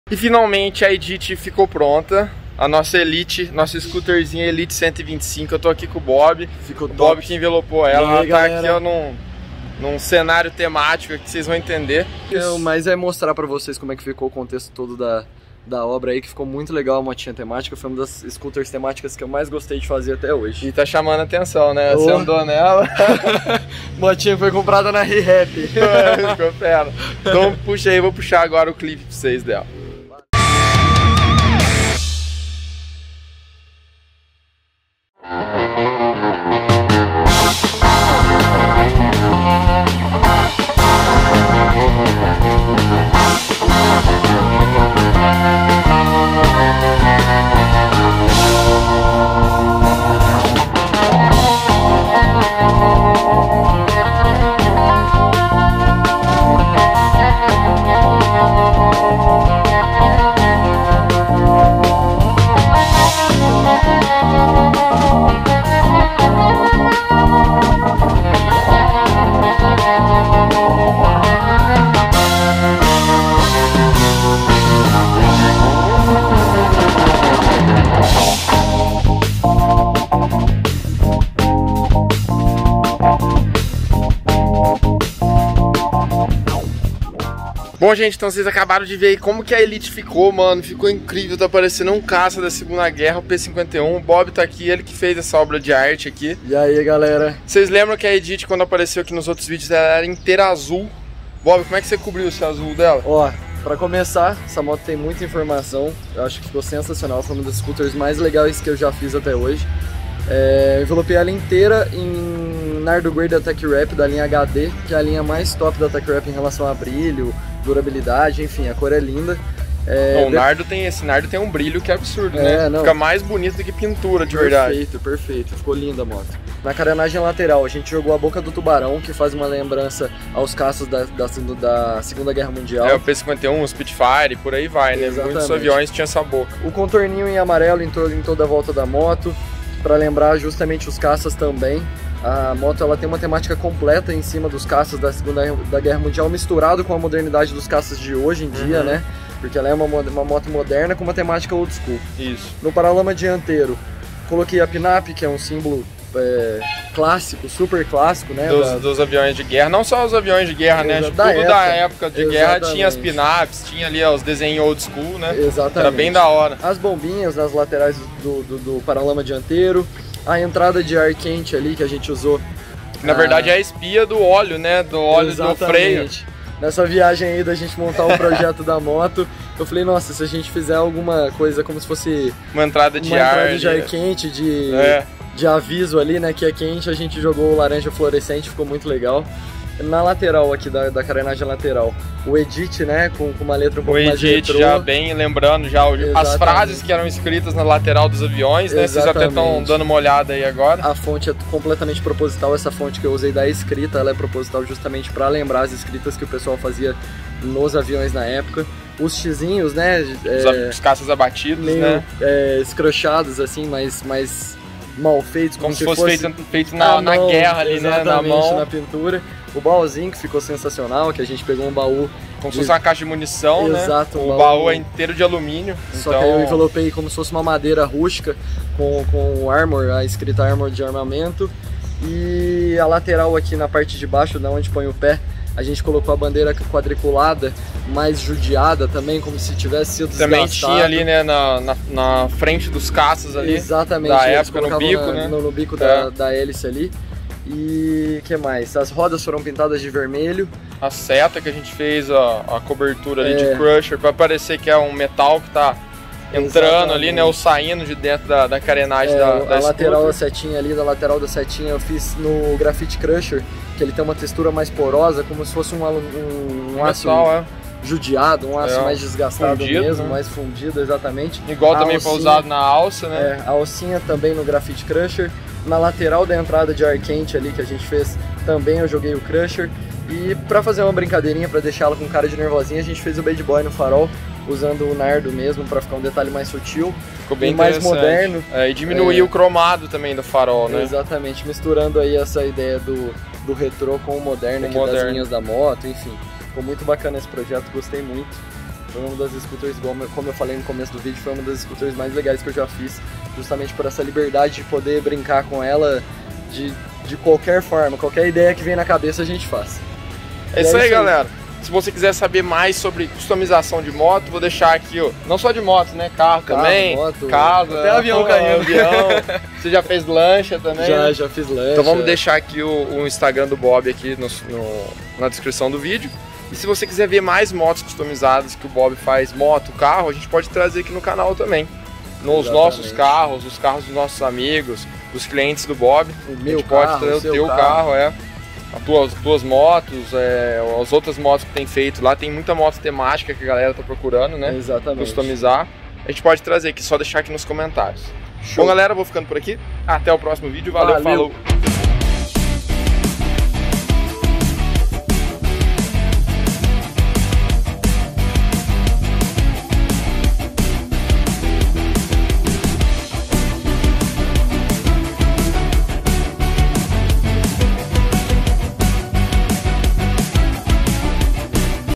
HONDA ELITE 125 CALL OF DUTY | SCOOTER CUSTOMIZADA | Rafael Togni
No vídeo de hoje você vai ver como minha Scooter Honda Elite 125 ficou no final da customização feita pelo

Outras informações:
Gravação: Samsung Galaxy S20 Ultra | Rafael Togni
Edição: Adobe Premiere 2020 | Rafael Togni
Capa/Thumbnail: Adobe Photoshop 2020 | Rafael Togni